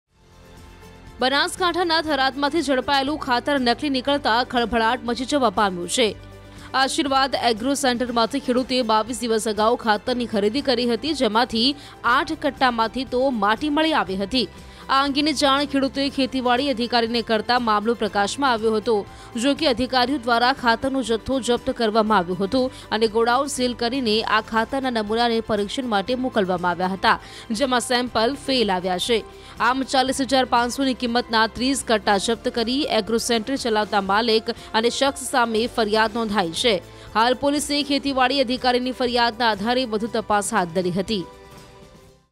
Banaskantha News: ભારે કરી! ખાતરના 30 કટ્ટા નકલી નીકળ્યા | Tharad | Fake Fertilizer | News18 | N18V
There has been a commotion due to fake manure seized from Banaskantha. The farmer had purchased fertilizer from Ashirwad Agro Center 22 days ago, out of which soil was found in 8 kattas.

બનાસકાંઠાના થરાદમાંથી ઝડપાયેલું ખાતર નકલી નીકળતા ખળભળાટ મચી જવા પામ્યો છે. આશિર્વાદ એગ્રો સેન્ટરમાંથી ખેડૂતે 22 દિવસ અગાઉ ખાતરની ખરીદી કરી હતી જેમાંથી 8 કટ્ટામાંથી તો માટી મળી આવી હતી. n18oc_banaskantha